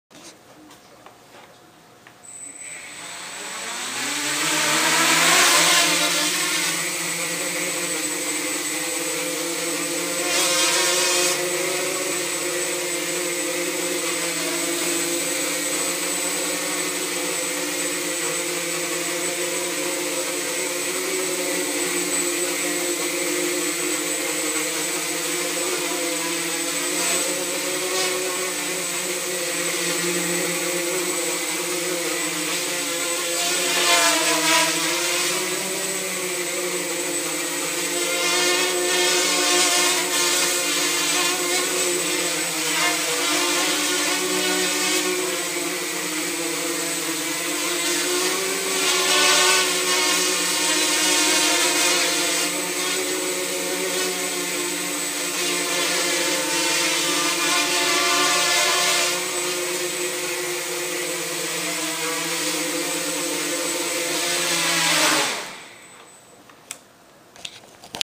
MultiWii Mini Quad
My mini quad running MultiWii 1preter7 and flying in stable mode. Using ITG3200 gyro, BMA180 accelerometer, HMC5883L magnetometer, atmega328 microcontroller running arduino bootloader. Using 10g brushless motor and 10A ESCs on 2Cell Lipo battery.